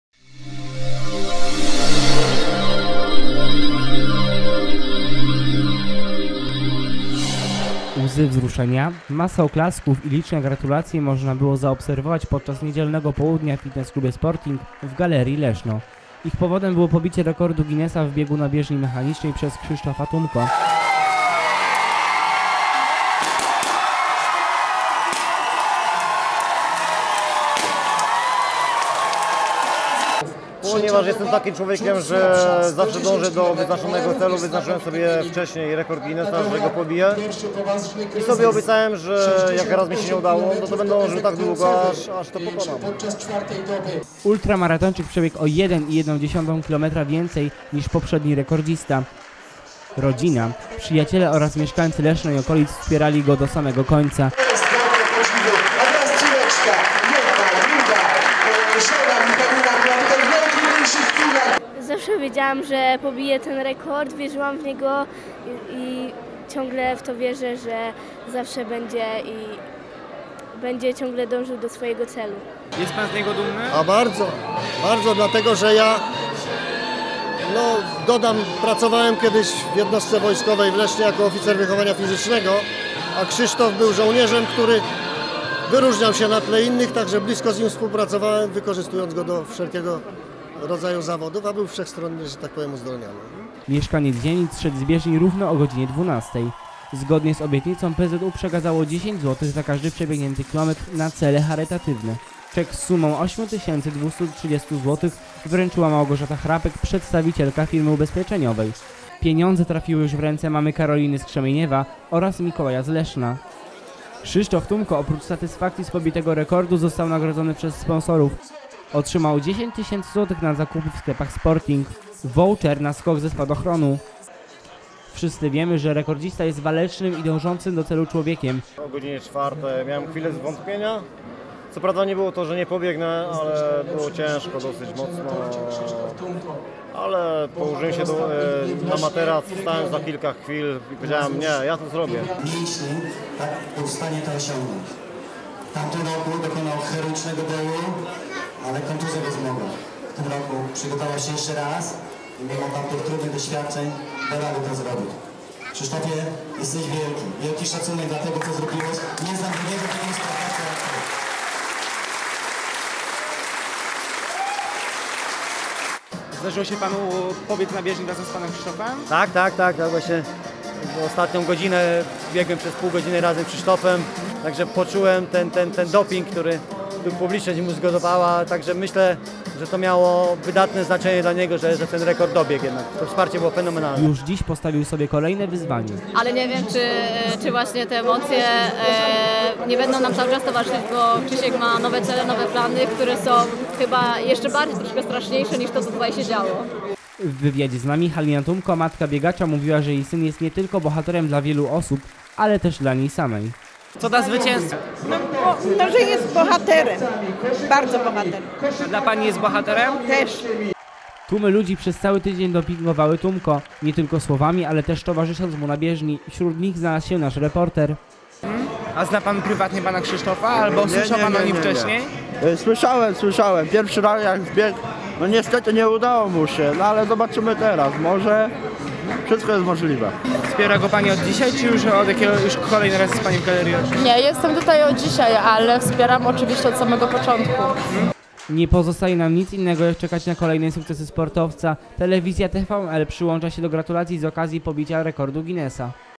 Tumko zakończył bieg z rekordem
Przez 7 dni Krzysztof Tumko zmagał się z czasem, bólem i samym sobą, a wszystko po to, aby pobić rekord Guinnessa. Ostatecznie się udało! Sportowiec zszedł z bieżni po 824,69 km. Tłumy ludzi gratulowały triumfu w Galerii Leszno. Nie obyło się też bez łez i kwiatów.